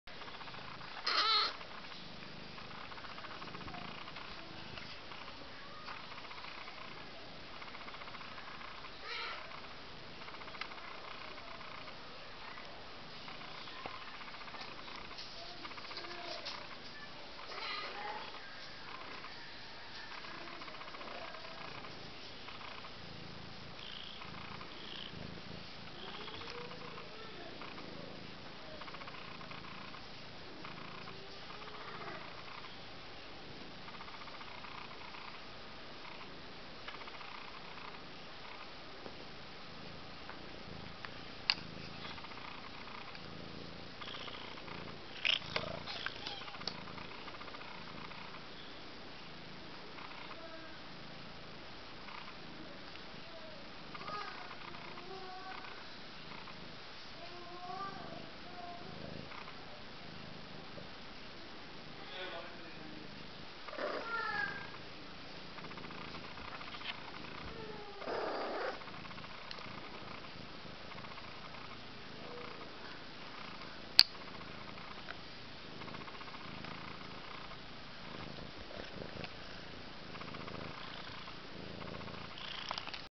Chatons abyssins du Vianey - 1 mois
Jolie Doll (lièvre) et Juno (fawn) à gauche
Jellybean (lièvre) et Jungle Boogie (sorrel) à droite